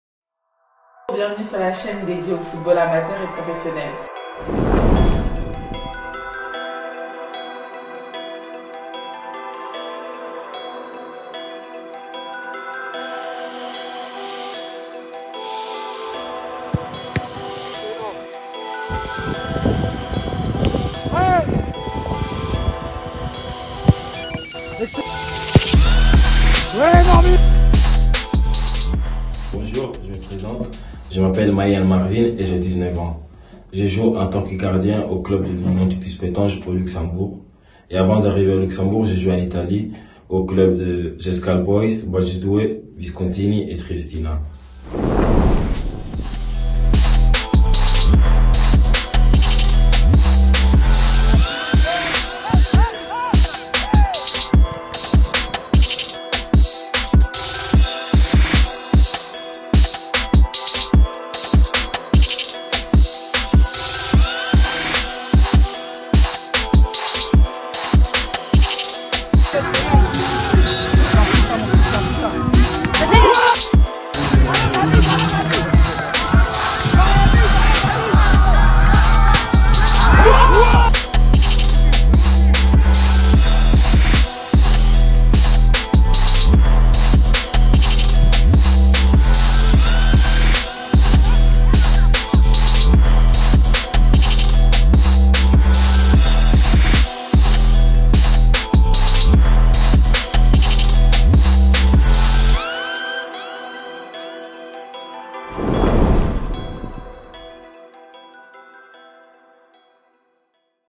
Yan Marvin MAHI Gardien de But de l'Union Titus Pétange - Je me présente - ESI
L'Union Titus Pétange est un club de football de Ligue1 (BGL) luxembourgeois basé à Pétange ⚽💪💪💪Yan Marvin MAHI